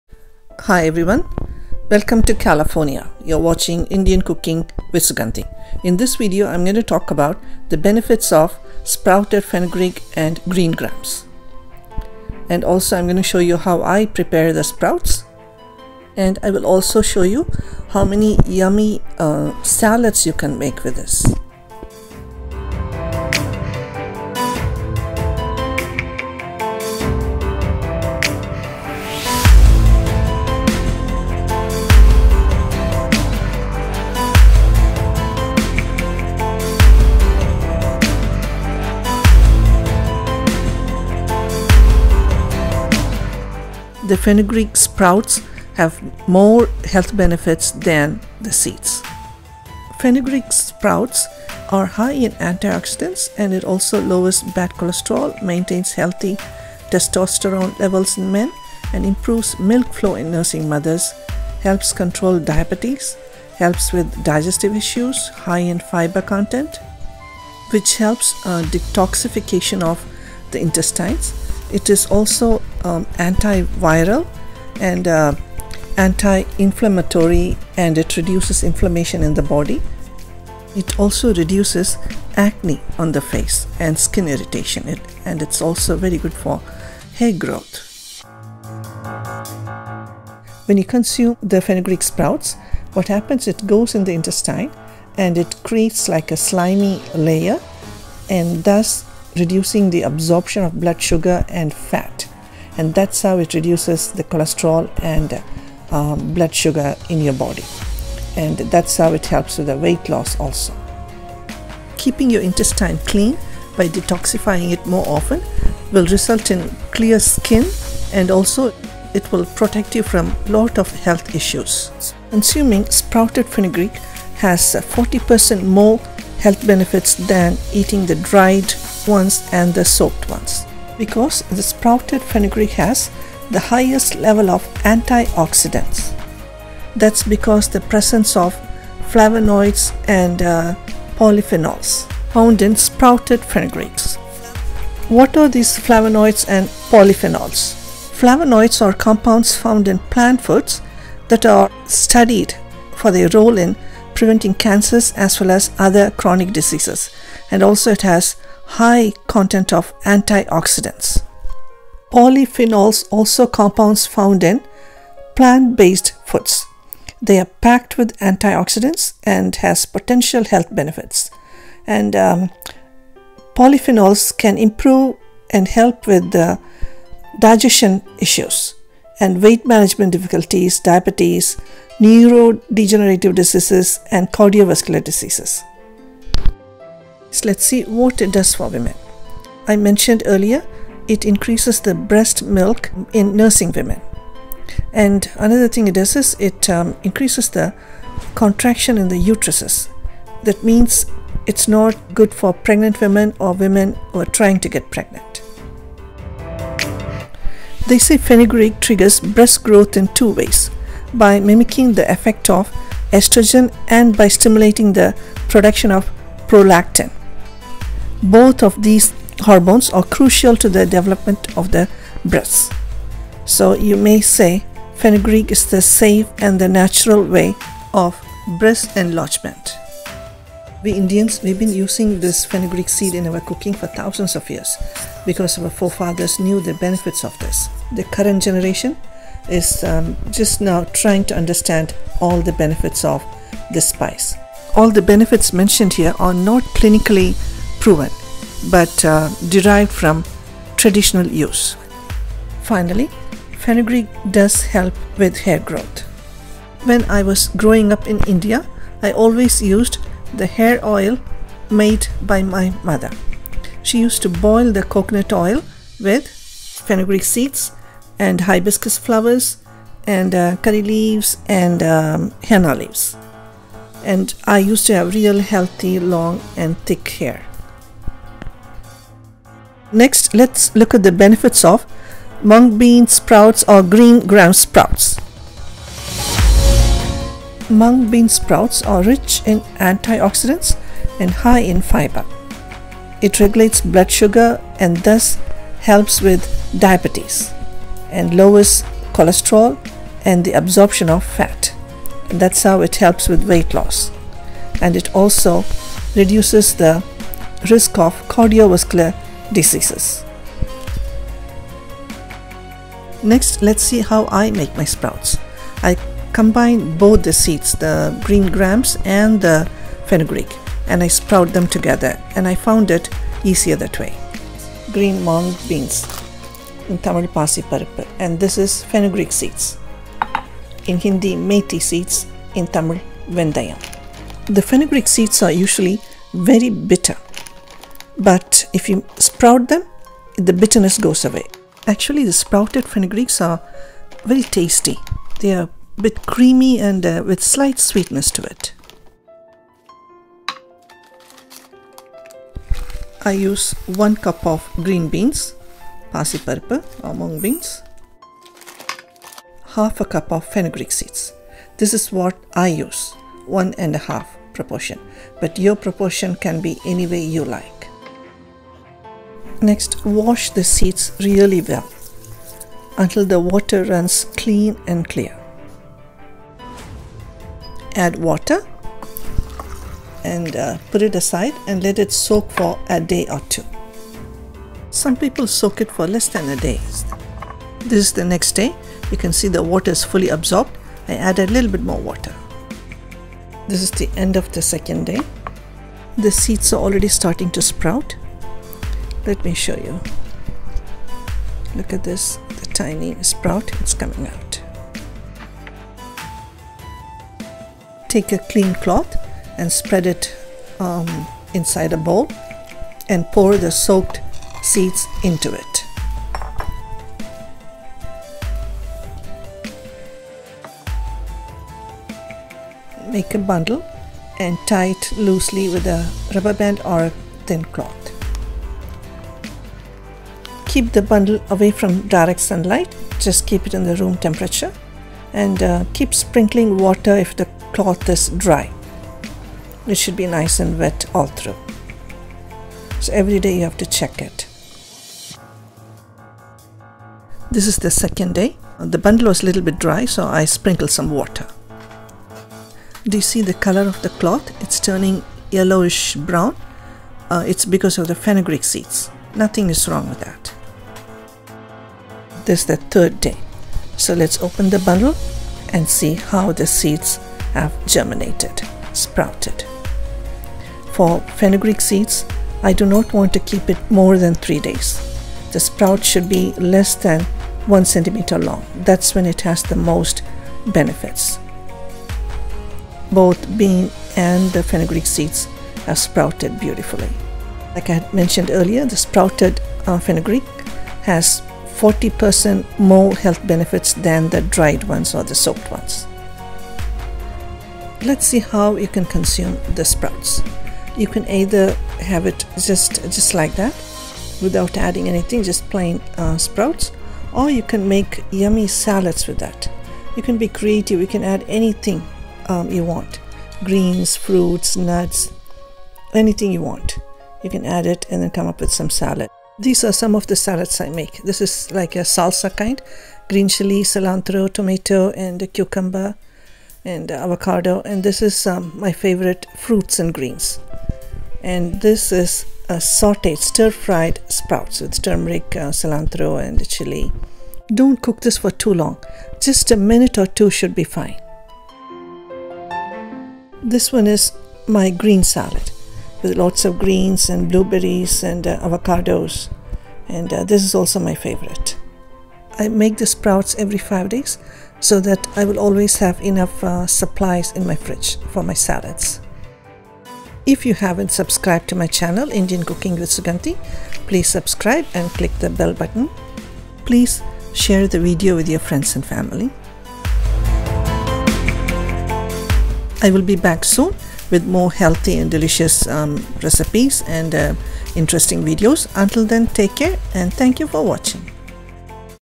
Fenugreek & mungbean Sprouts,antioxidants, lowers diabetes& cholesterol, increases testosterone
Amazing health benefits of sprouted fenugreek & moong beans.  These sprouts are high in antioxidants, & high in fiber, excellent detoxifier, lowers blood sugar & bad cholesterol, cures acne, helps with  hair growth and stimulates growth in  breast tissue in women.

You will see in this video how many ways you can consume these sprouts including
ideas for many yummy salads and stir fry.

Also visit my FB page: @suganthisTastycuisine